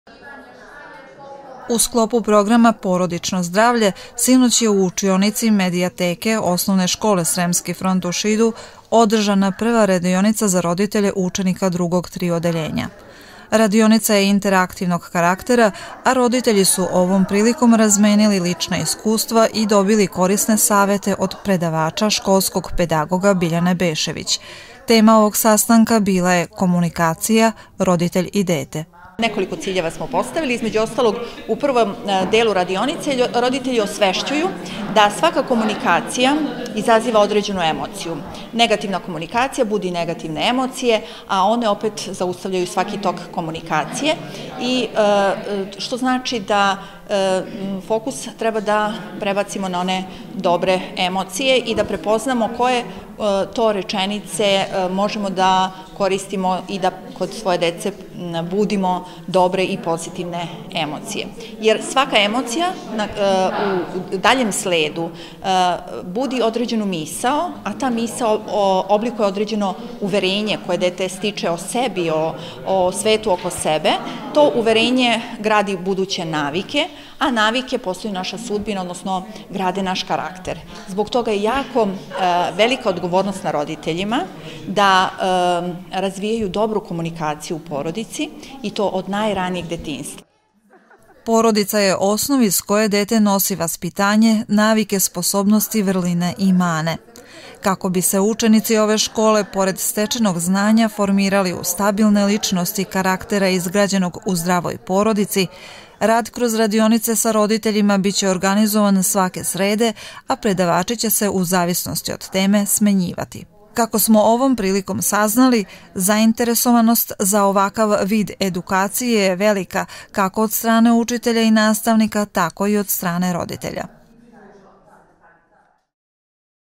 Radionica roditelja OS Sremski front
Radionica za roditelje pod nazivom „Komunikacija roditelj i dete“  održana je u Osnovnoj školi „Sremski front“ u Šidu. Radionicu je vodila Biljana Bešević,školoski pedagog, a slični edukativni sastanci biće organizovani i u narednom periodu